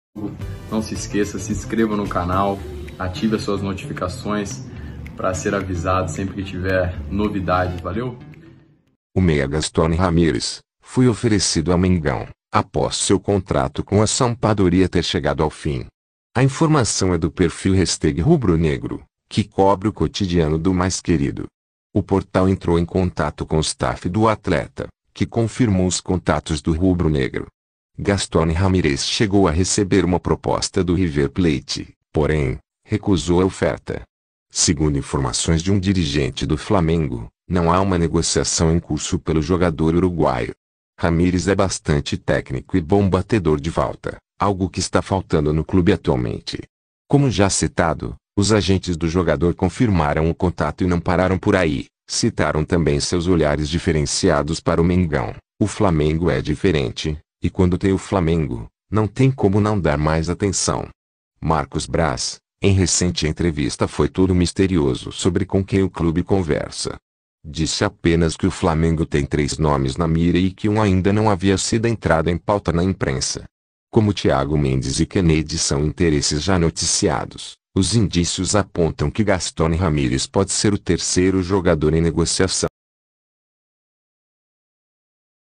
Gastón Ramírez foi oferecido ao Flamengo
Antigo desejo de Renato encerra vínculo na Itália e entra em pauta na Gávea: “Fla é diferente”, afirma staff

Jogador já recusou proposta do River Plate e agentes dão destaque para o Mengo: “Quando tem o Flamengo, não tem como não dar mais atenção“

O Fla se movimenta no mercado em busca de um meia. A meia-cancha Rubro-Negra carece de reforços após a saída de Gerson, entretanto, uma possibilidade foi posta à mesa de Marcos Braz, vice-presidente de futebol da Gávea. Trata-se do meia Gastón Ramírez, que foi oferecido ao Mengão, após seu contrato com a Sampdoria-ITA ter chegado ao fim. A informação é do perfil Hashtag Rubro-Negro, que cobre o cotidiano do Mais Querido.

A negociação, se for aberta, tem um grande aliado para que a transferência seja concretizada. O treinador Renato Gaúcho, quando estava no Grêmio, chegou a pedir a contratação do jogador. O portal entrou em contato com o staff do atleta, que confirmou os contatos do Rubro-Negro.

Gastón Ramirez chegou a receber uma proposta do River Plate, porém, recusou a oferta. Segundo informações de um dirigente do Fla, não há uma negociação em curso pelo jogador uruguaio. Ramírez é bastante técnico e bom batedor de falta, algo que está faltando no clube atualmente.

Como já citado, os agentes do jogador confirmaram o contato e não pararam por aí, citaram também seus olhares diferenciados para o Mengão: “O Flamengo é diferente, e quando tem o Flamengo, não tem como não dar mais atenção“. Marcos Braz, em recente entrevista foi todo misterioso sobre com quem o clube conversa. Disse apenas que o Fla tem três nomes na mira e que um ainda não havia sido entrado em pauta na imprensa. Como Thiago Mendes e Kenedy são interesses já noticiados, os indícios apontam que Gastón Ramírez pode ser o terceiro jogador em negociação.